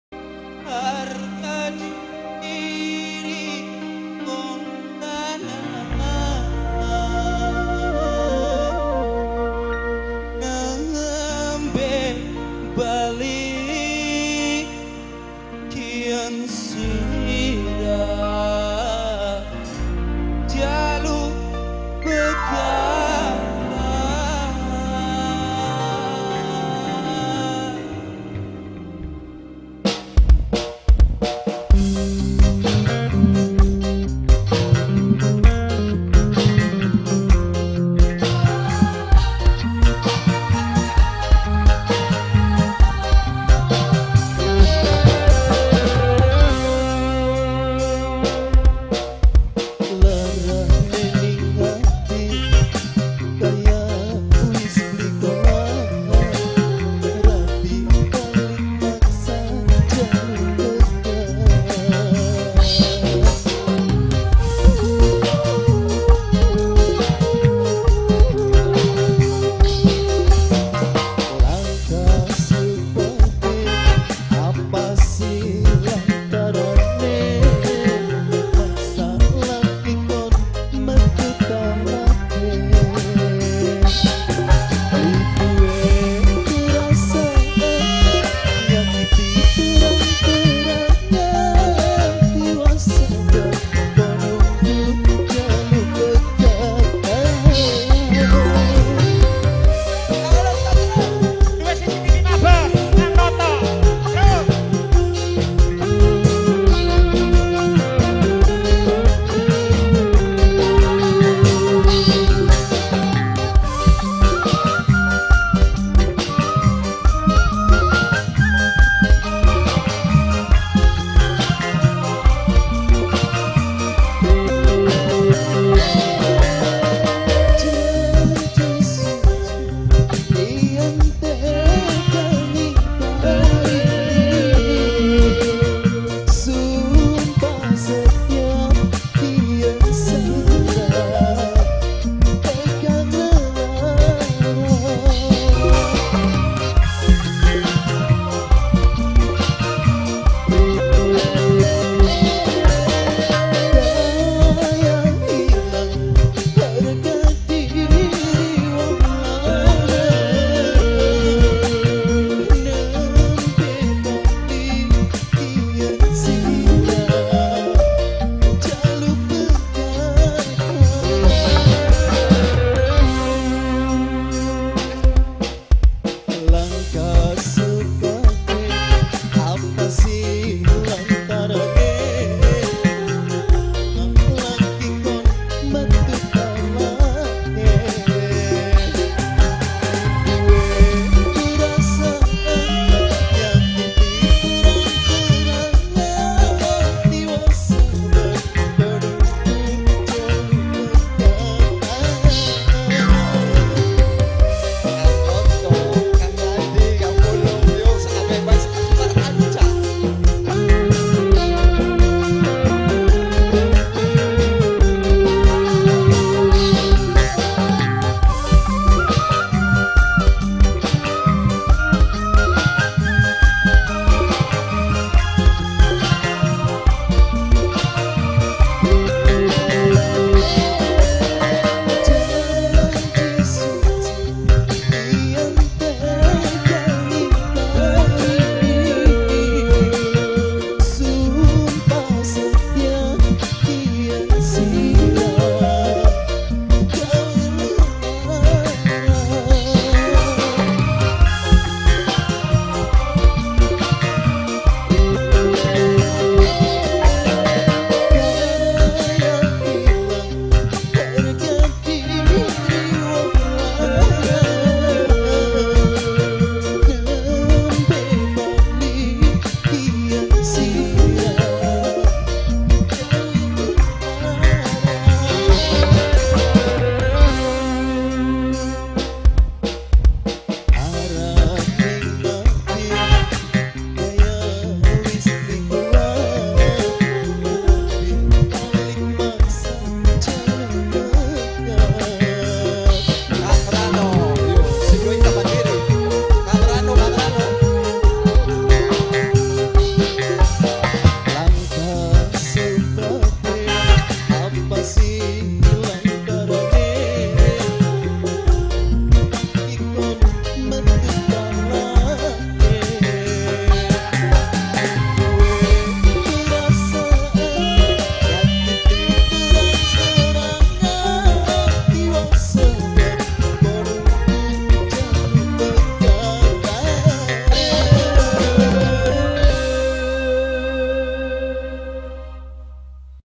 ANDI PUTRA 1 METU TALAKE DESA DRUNTEN WETAN BLOK KARANGASEM KEC GABUS WETAN INDRAMAYU
KHITANAN ADITYA  KEL BAPAK SUKANA IBU NIA DESA DRUNTEN WETAN BLOK KARANGASEM KEC GABUS WETAN INDRAMAYU